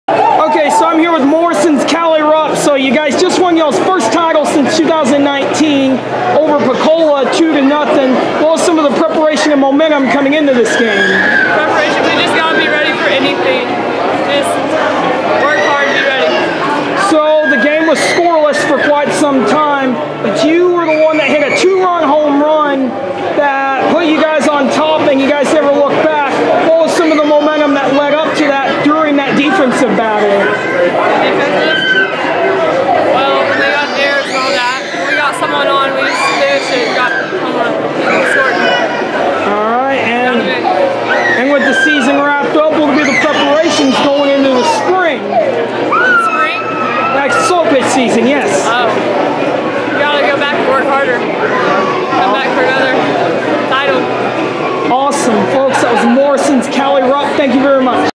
Interview with Kallie Rupp of Morrison
I interview Morrison shortstop Kallie Rupp after the team's 2-0 win over Pocola in the Class 2A State Championship